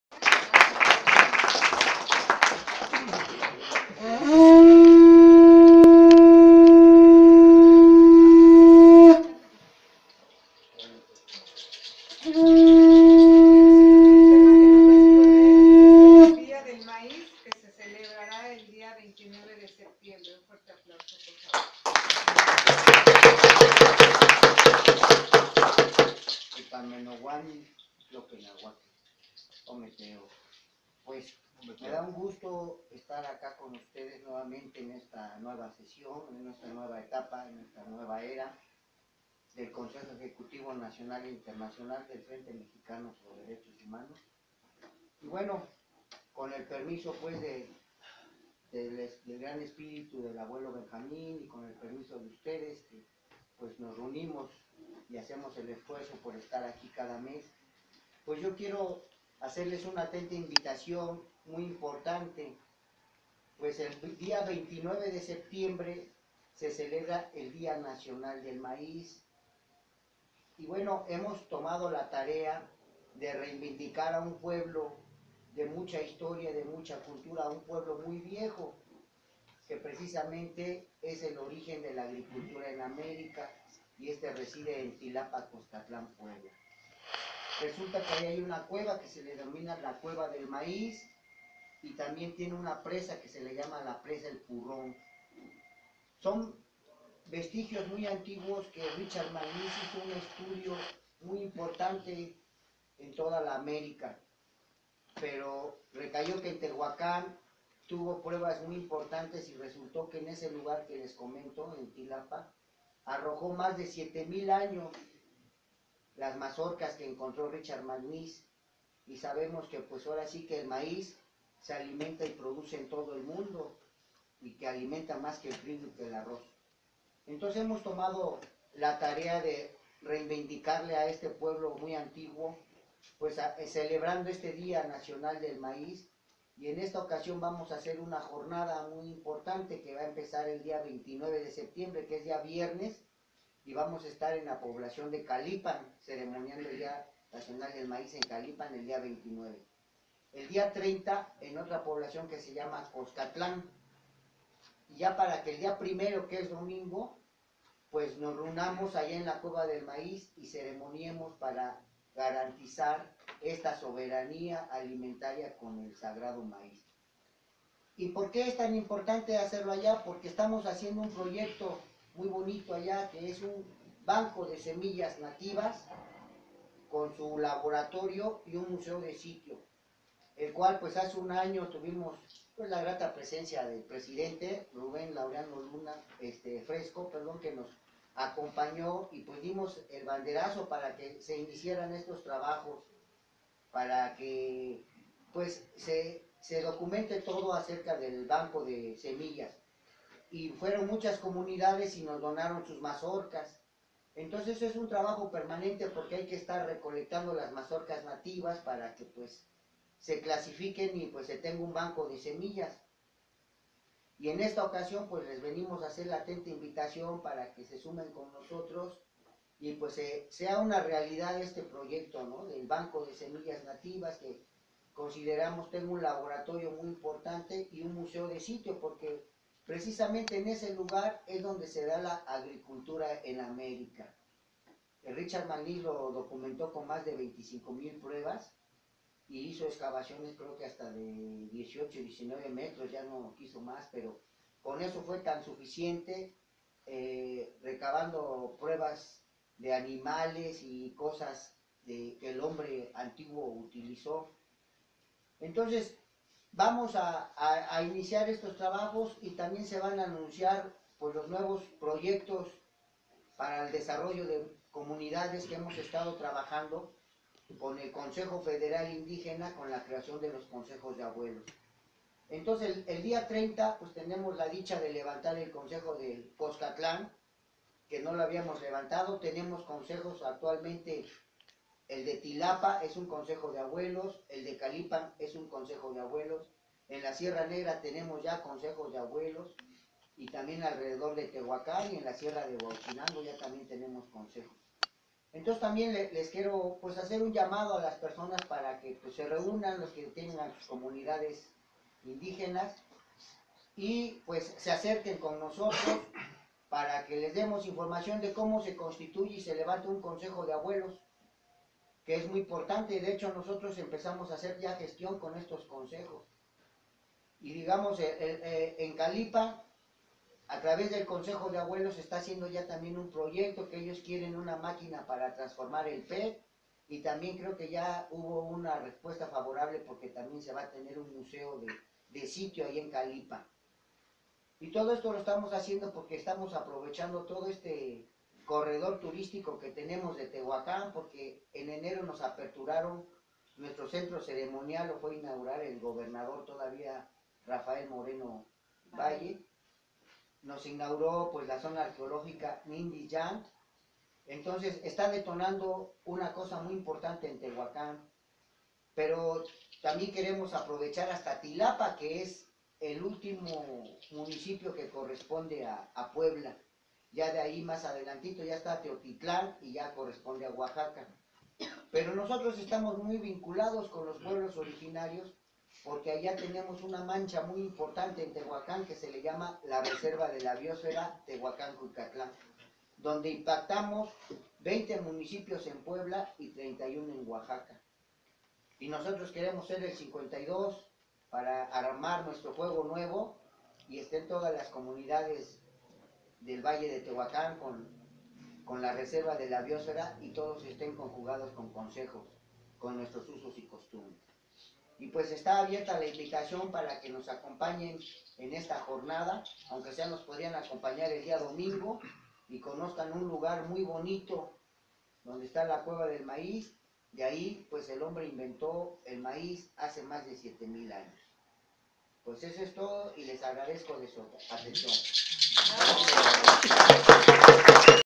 Parlamento de Derechos Humanos Benjamin Laureano Luna. Asamblea legislativa.
Consejo federal Indígena.
Jorge Renato Negrete Cedillo.
Celebración del Maíz. Jornada 29 de Septiembre Calipan. 30 Coxcatlan y 1 de Octubre En la Cueva del Maíz. tilapa.